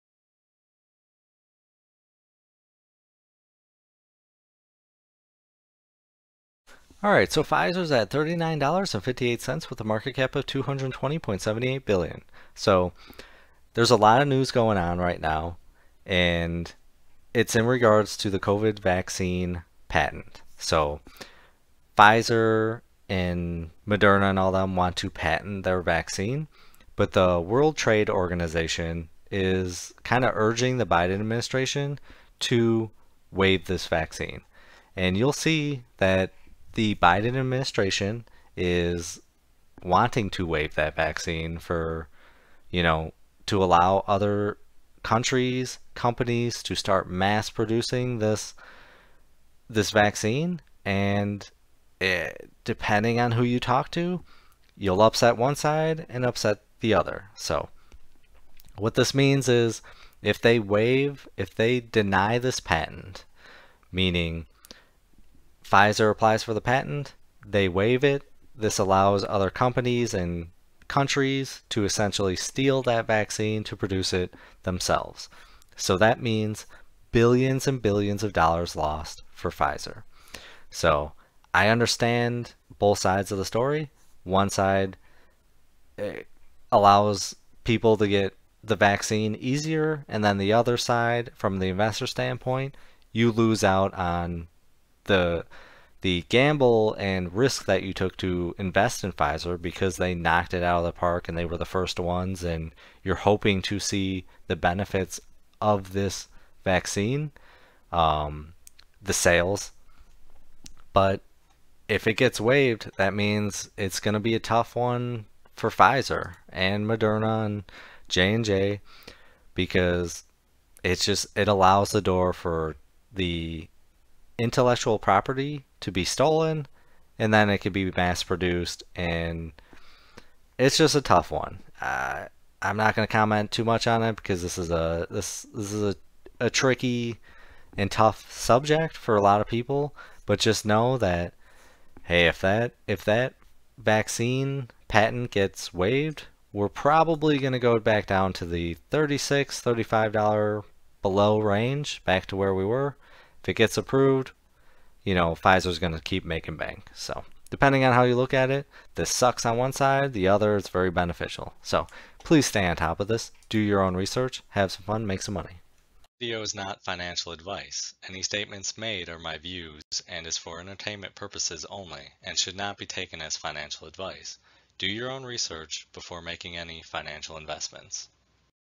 Pfizer Stock(PFE) - Will Biden Administration Cost Pfizer Billions of Dollars?
PFE Stock
Pfizer Stock